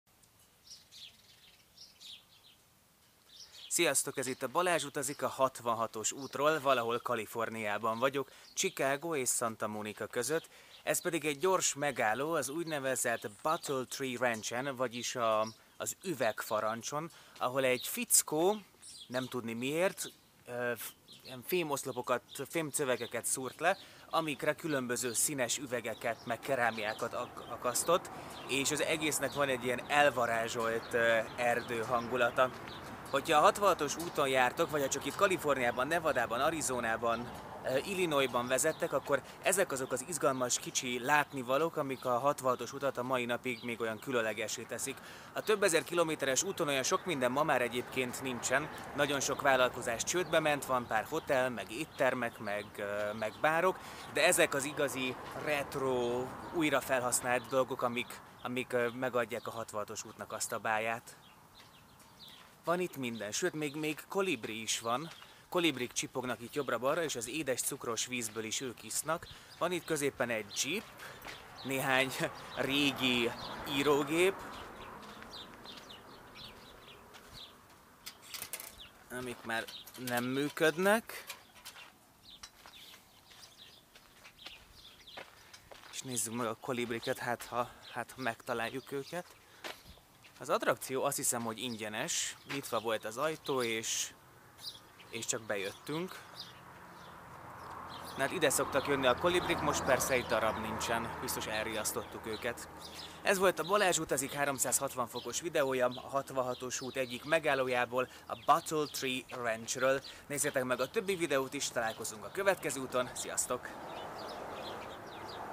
Üvegfa erdő a 66-os úton - VR 360
Ha a 66-os úton a vezetés közben meg akarsz pihenni, ide mindenképp térj be a kaliforniai szakaszon. Az üvegfa ranchon (bottle tree ranch) egy elvarázsolt üvegerdőben tudsz "túrázni".

Ilyen és hasonló újrahasznosított látnivalókkal vannak a legendás autós úton Chicago és Santa Monica között.